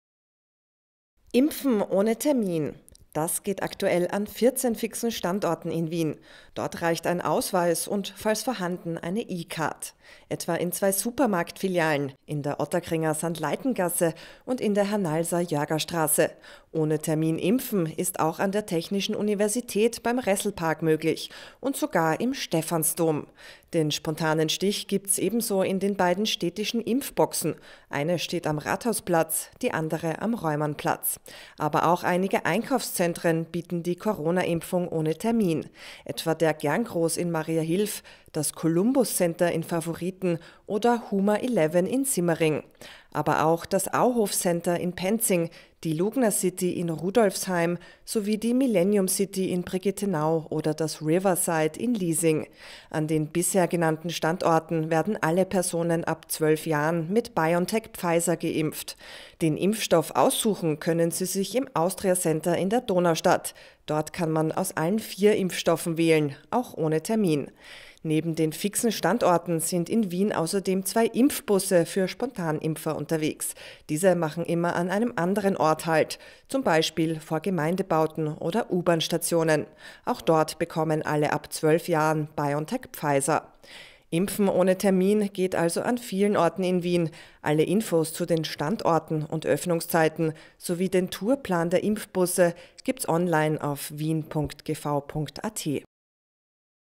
Wo kann ich mich im harten Lockdown in Wien ohne Voranmeldung impfen lassen?
Alessa Däger hat die aktuellen Impf-Standorte der Bundeshauptstadt - ohne Voranmeldung - zusammengefasst!